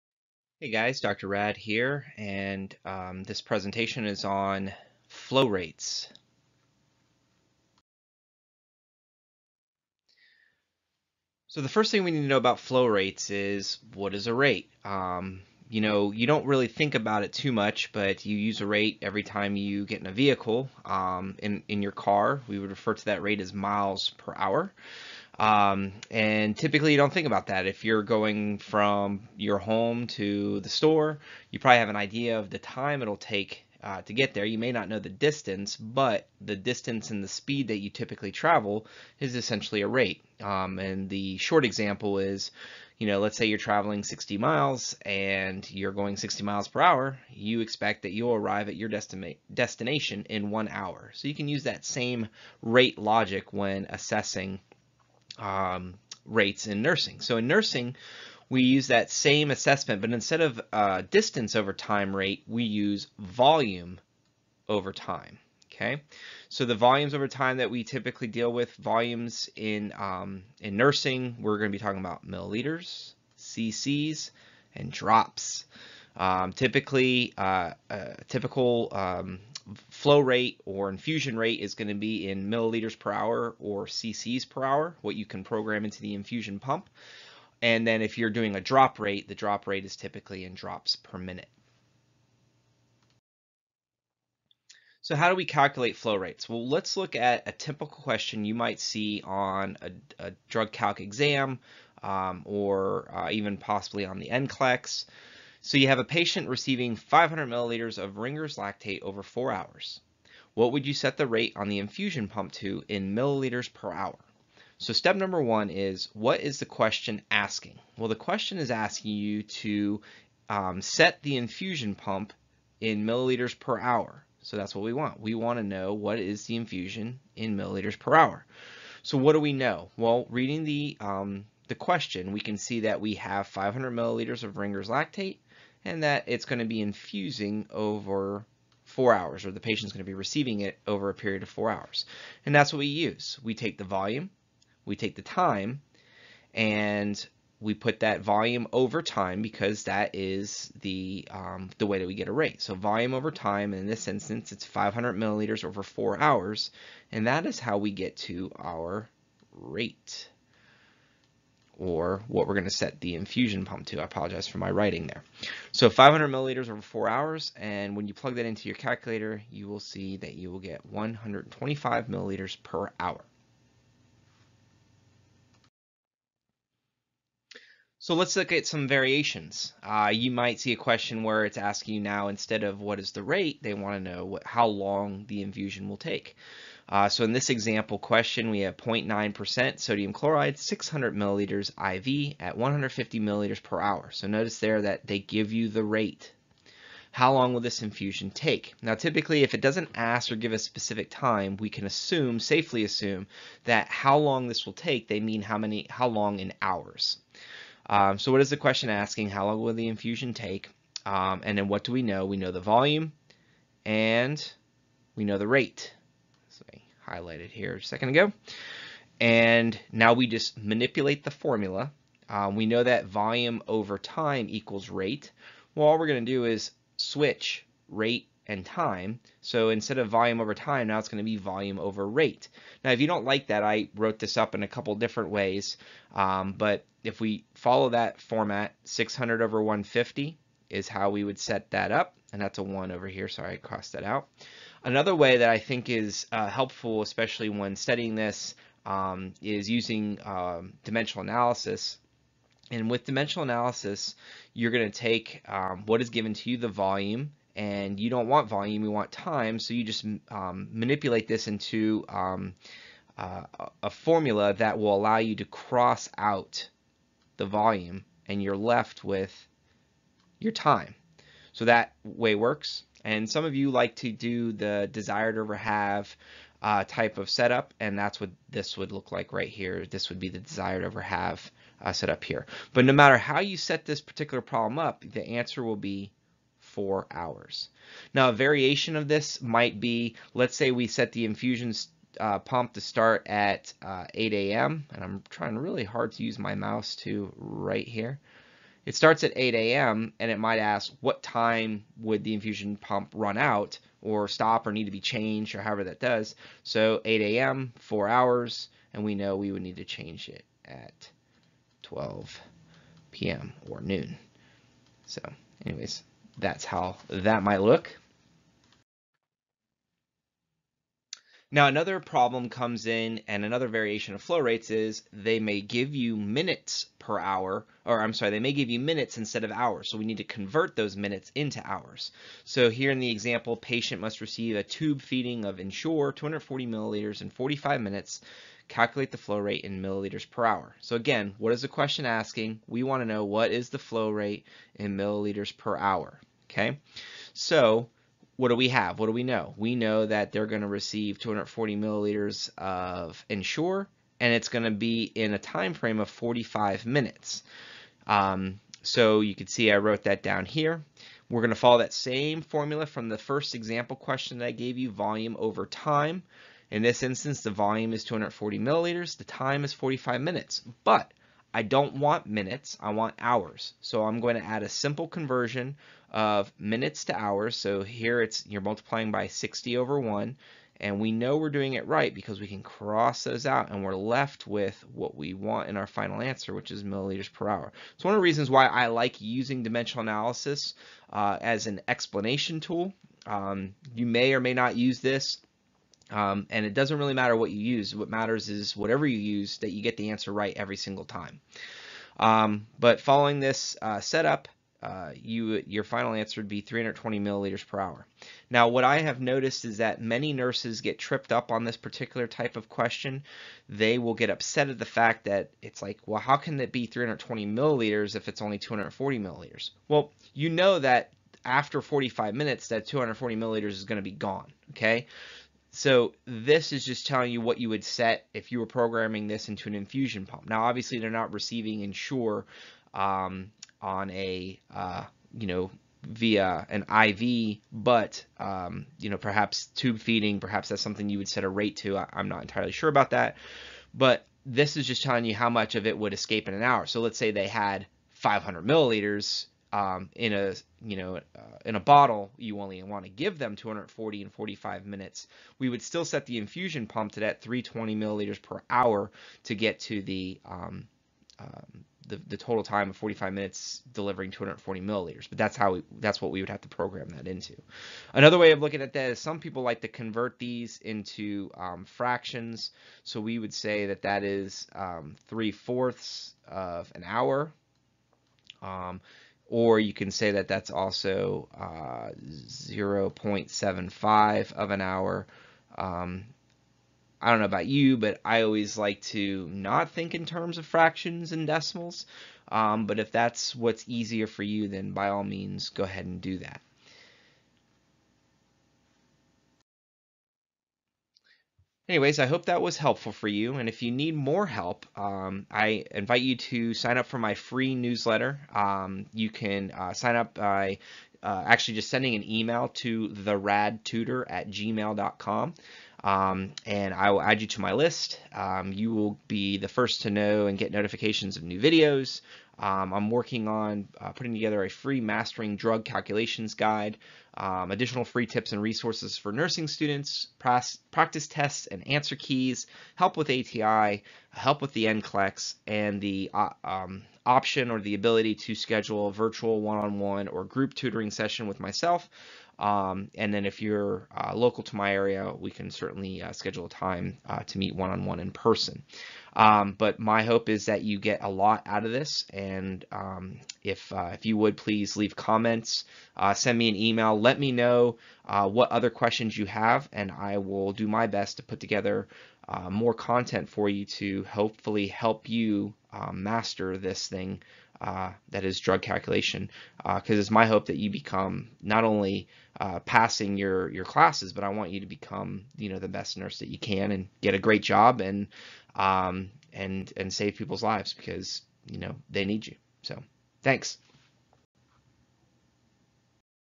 Drug Calculations for Nursing Students - Flow Rates
Basic review of calculating flow rates for nursing students. Want FREE help with drug calculations?